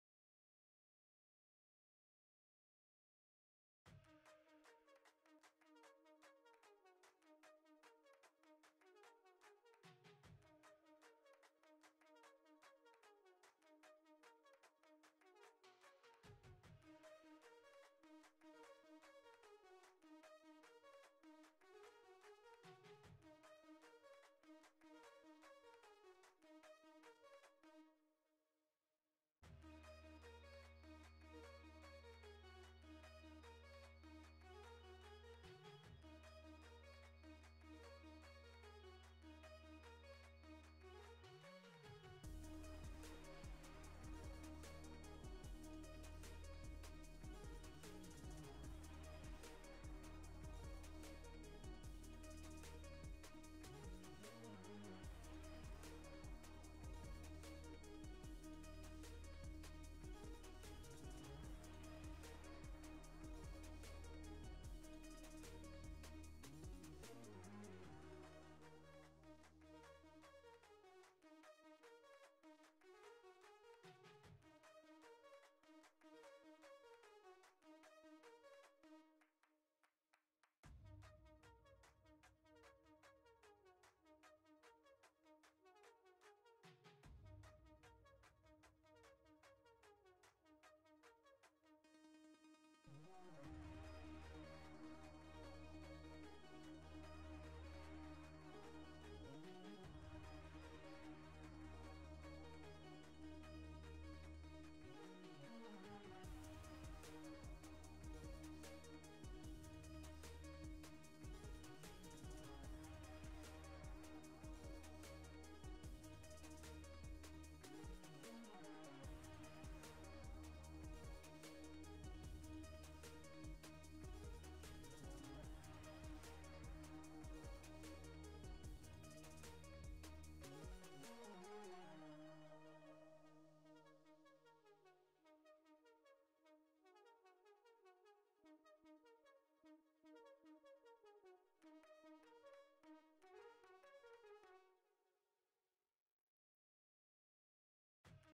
Unedited Dance Session
I made this video to try my hand at video editing, but I haven't found a good app for PC to add cool effects.